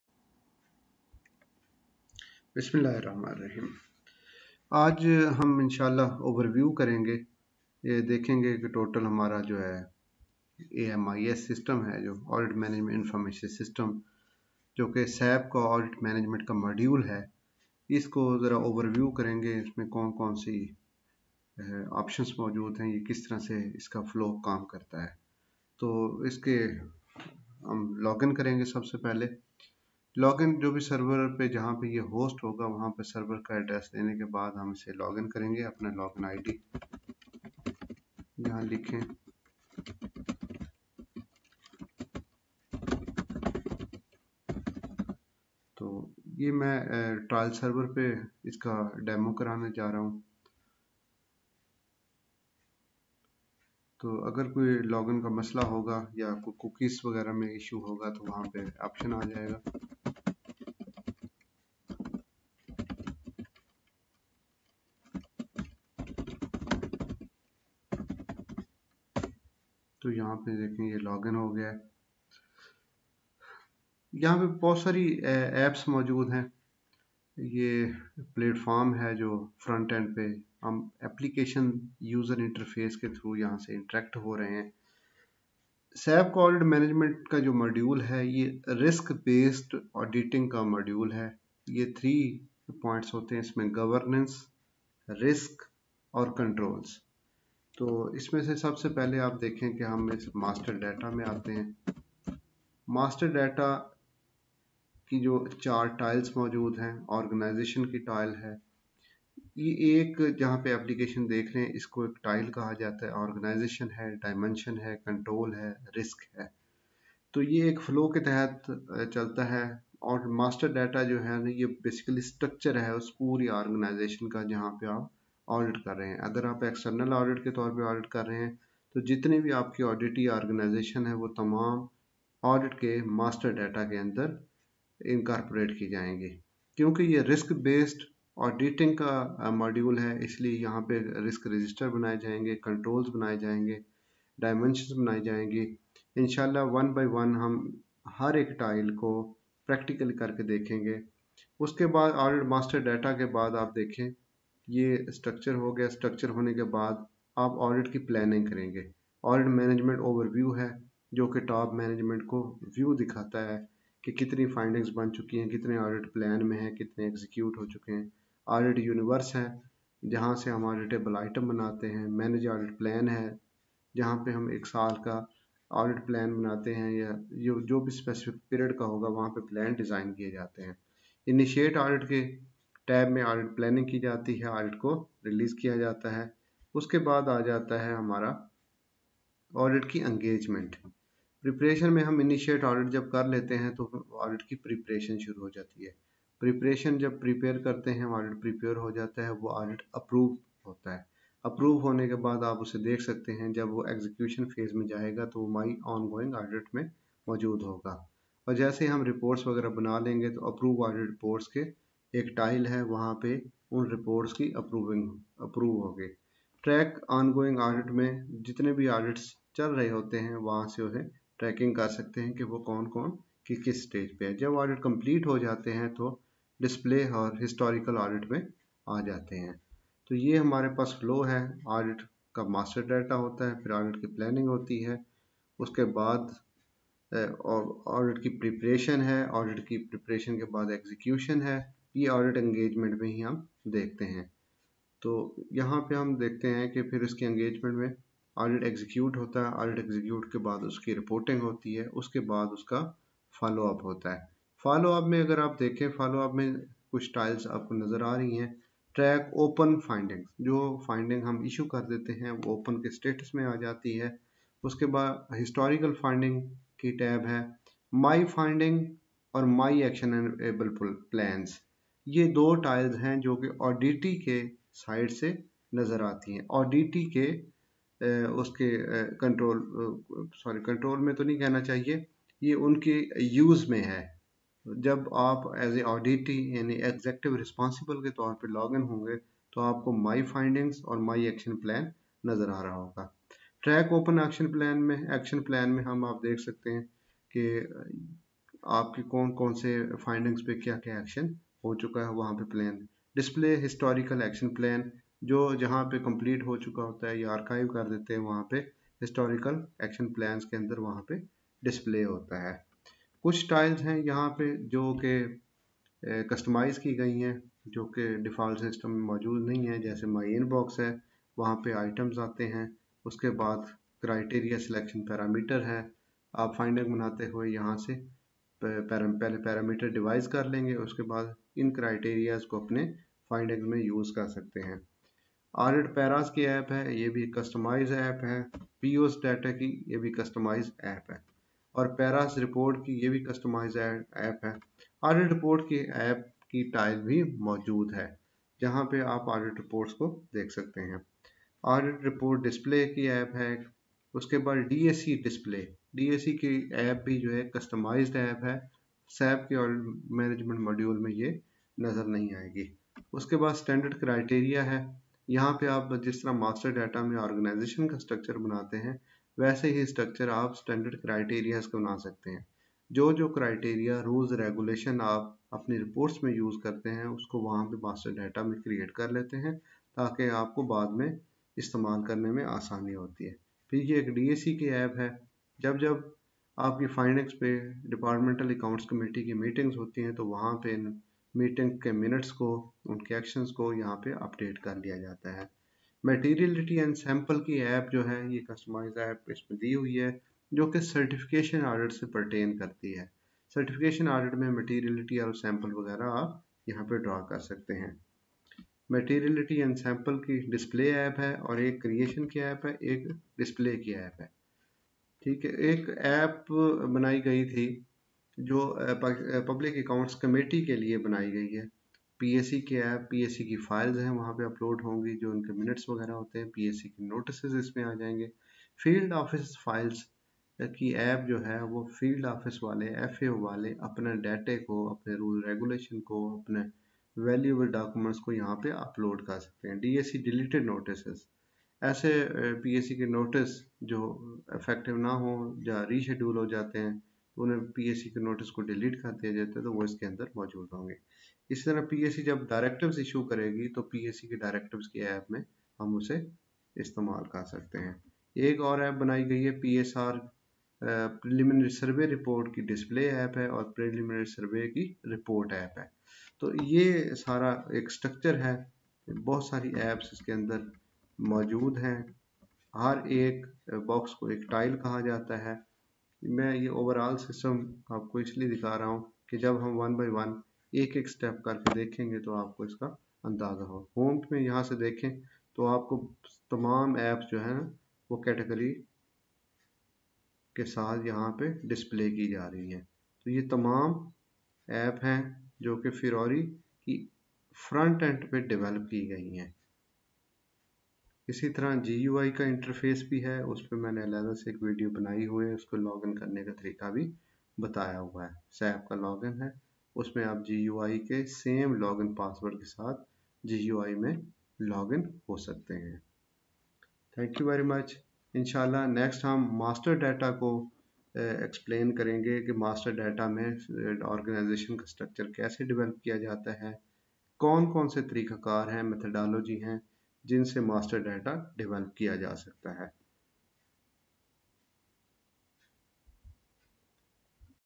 overview of AMIS SAP Audit Management
overview of sap audit management system AMIS introduction of front end firori tiles tabs and application used in front end SAP Audit management System. AMIS in AGP use the front end with application roles to interact with SAP Audit Management Information System.